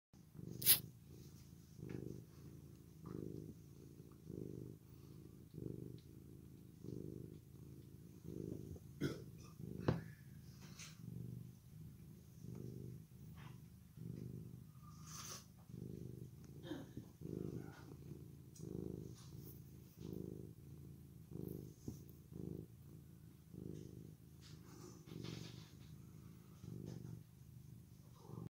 Orange Tabby Cat Looks Like Donald Trump With Toupee
I brushed my cat the other day and when I was finished I put his fur on top of his head like a toupee. Watch the video for the end result!